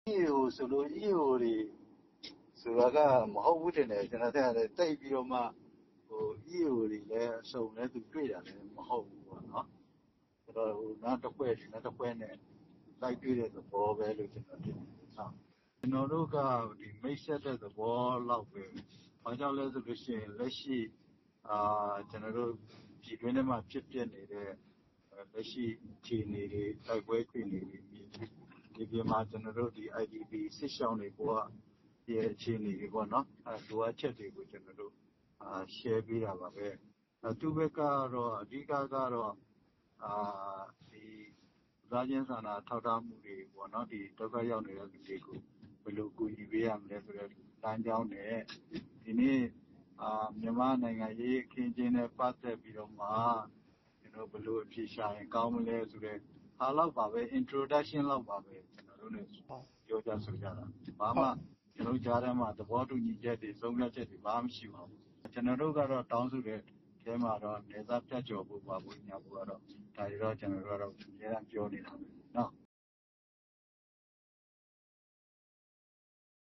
ထိုင်းဝန်ကြီးချုပ်ဟောင်း သက်ဆင်နှင့် တွေ့ဆုံခဲ့မှု ဘာတွေထူးခြားလဲ
ပြီးခဲ့သည့်လ(ဧပြီ) က ထိုင်းနိုင်ငံ၊ ချင်းမိုင်မြို့တွင် အမျိုးသားညီညွတ်ရေးအစိုးရ (NUG) အပါဝင် တော်လှန်ရေး အင်အားစုများကို ထိုင်းဝန်ကြီးချုပ်ဟောင်း သက်ဆင်ရှင်နာဝပ်က တွေ့ဆုံဆွေးနွေးခဲ့သည်။

"ပြည်တွင်းထဲမှာဖြစ်နေတဲ့ လက်ရှိအခြေအနေတွေ၊ တိုက်ပွဲအခြေအနေတွေ၊ ‌စစ်ရှောင်တွေဘဝ အခြေအနေတွေပေါ့။ လိုအပ်ချက်တွေကို ကျွန်တော်တို့ Share(မျှဝေ)ပေးတာပါပဲ။ သူ့(သက်ဆင်)ဘက်ကတော့ လူသားချင်းစာနာထောက်ထားမှုတွေ၊ ဒုက္ခရောက်နေတဲ့သူတွေကို ဘယ်လိုကူညီပေးရမလဲ ဆိုတဲ့ လမ်းကြောင်းနဲ့ မြန်မာ့နိုင်ငံရေးအခင်းအကျင်းနဲ့ပတ်သက်ပြီး ဘယ်လိုအဖြေရှာရင်ကောင်းမလဲဆိုတဲ့ Introduction(မိတ်ဆက်)သဘောလောက်ပါပဲ" ဟု တွေ့ဆုံမှုနှင့်ပတ်သက်ပြီး အမျိုးသားတိုးတက်ရေးပါတီ(KNPP)မှ အတွင်းရေး ၂ ဖြစ်သူ ဦးအောင်ဆန်းမြင့်က ပြောသည်။

လက်ရှိဖြစ်ပေါ်နေသည့် မြန်မာအရေးနှင့်ပတ်သက်၍ ဝန်ကြီးချုပ်ဟောင်းသက်ဆင်နှင့် ကိုယ်စားလှယ်အဖွဲ့က  အာဏာသိမ်းစစ်ကောင်စီကို ခုခံတိုက်ခိုက်နေသည့် တိုင်းရင်းသားလက်နက်ကိုင်များ၊ NUGနှင့် တော်လှန်ရေးအင်အားစုများကို ဧပြီ ၁၃ ရက်တွင် တွေ့ဆုံဆွေးနွေးခဲ့ခြင်းဖြစ်သည်။

သက်ဆင်နှင့် တွေ့ဆုံမှုတွင် ကရင်အမျိုးသားအစည်းအရုံး (KNU)၊ ကရင်နီအမျိုးသားတိုးတက်ရေးပါတီ(KNPP)၊ ကချင်အမျိုးသားအစည်းအရုံး (KNO)၊ NUG စသည့် တော်လှန်ရေးအင်အားစုများပါဝင်ခဲ့သည်။